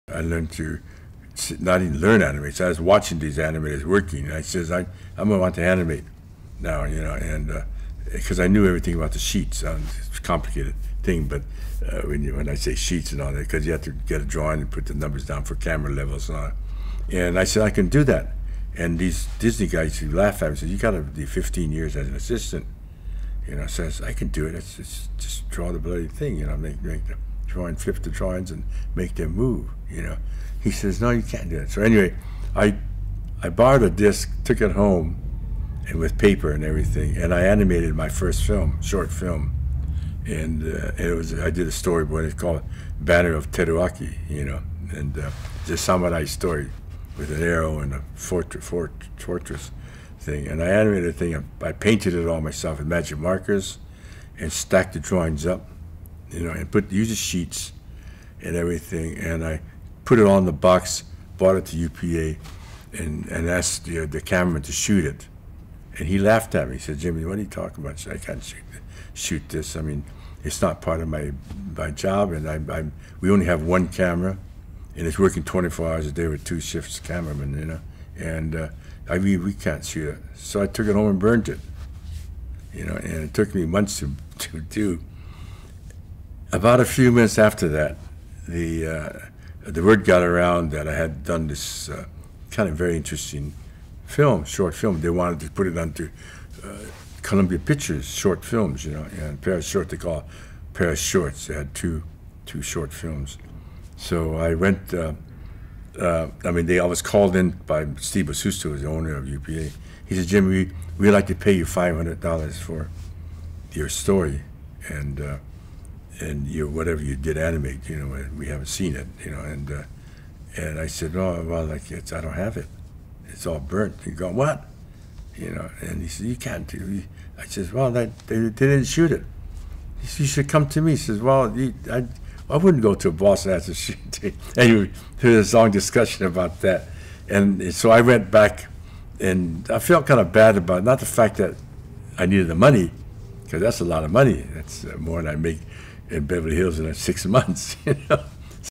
Jimmy Murakami interview #8: First Short Film
Interview Date: June 29, 2012
Location: Los Angeles, CA
Interviewers: Chris Komai, John Esaki
Camera: Akira Boch

Transcript:

I learned to, well I didn't learn to animate. I was watching these animators working. I says I uh wanna learn to animate now, you know, and uh, 'cause I knew everything about the sheets.  It's a complicated thing. But when I say sheets and all that cause I mean I had to get the drawing and get all the numbers down for camera levels and so on. And so I said, "I can do that."  And so these Disney guys who laughed at me said I gotta have at least 15 years as an assistant.  You know I says, "I can do it."  Just, just draw the bloody thing, you know, just draw the drawings and make them move. You know, he says, "No you can't do that."  So anyway I borrowed a disc, took it home, and with paper and everything,  and I animated my first film, my first short film.  And it was, uh, I did the storyboard and called it "Banner of Teruaki," you know.  And uh this samurai story with an arrow and a for-fortress thing.  And I animated the thing and I painted it all myself with Magic Markers and stacked the drawings up, you know, and put...used the sheets and everything.  And I put it on the box and brought it to UPA and asked the camera to shoot it.  And he laughed at me.  He says, "Jimmy, what are you talking about? I can't, I can't shoot this.  I mean, it's not part of my job and I'm, I'm...we only have one camera and it's working 24 hours a day with two shifts of cameramen, you know?  And I, uh, we can't shoot that."  So I took it home and burned it.  You know?  And it took me months to, to do.

About a few minutes after that the word got around that I had done this kind of interesting film, short film.  They wanted to put it onto Columbia Pictures' short films, you know, what they call a pair of shorts-- two, two short films. I was called in by Steve Azusu, who was the owner of UPA.  He said "Jim, Jim we'd like to pay you $500 for your story and whatever you did, animated.  You know we have a scene"  And I said, "You know, well, I can't. I don't have it.  It's all burnt."  He goes, "What!?" You know?  And he says, "You can't do..."  And I says, "Well he didn't shoot it." He says, "You should come to me," he says.  I say, "Well I wouldn't come to a boss and ask him to shoot a tape.